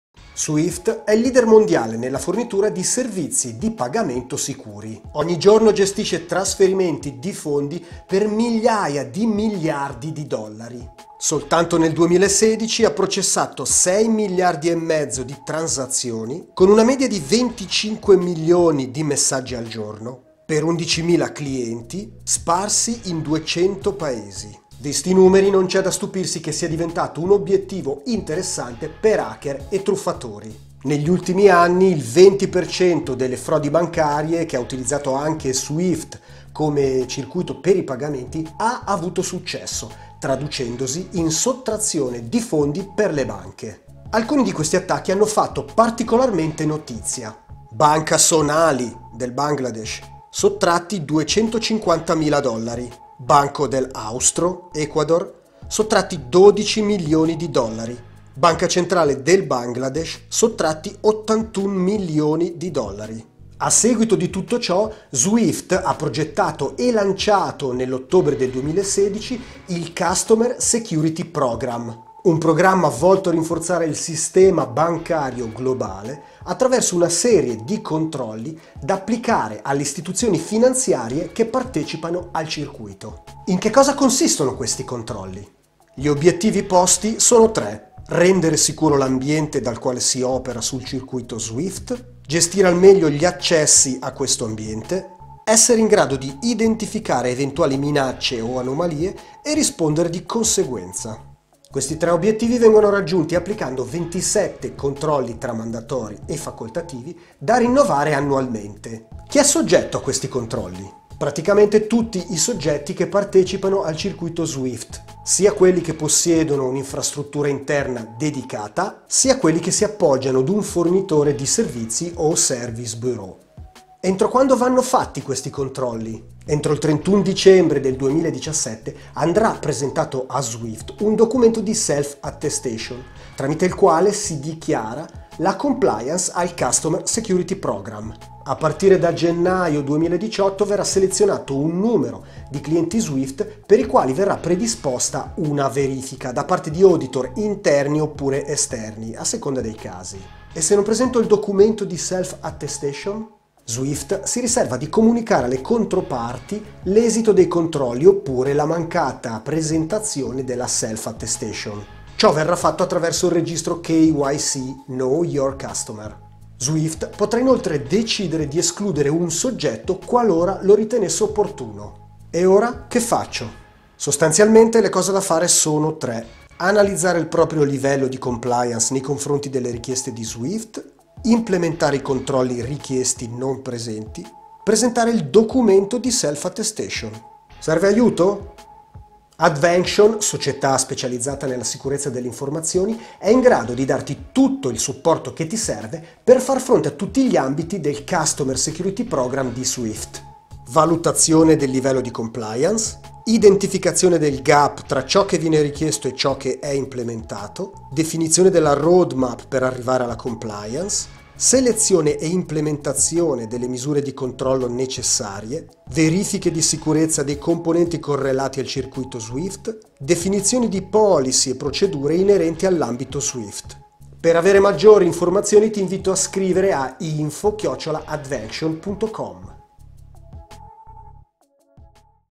SWIFT Customer Security Programme
Entro il 31 dicembre 2017 chiunque utilizzi il circuito SWIFT deve adempiere ad una serie di controlli imposti da SWIFT stessa.

In questo video viene spiegato in che cosa consiste il Customer Security Programme e come Advanction è in grado di fornire tutto il supporto necessario per essere compliant.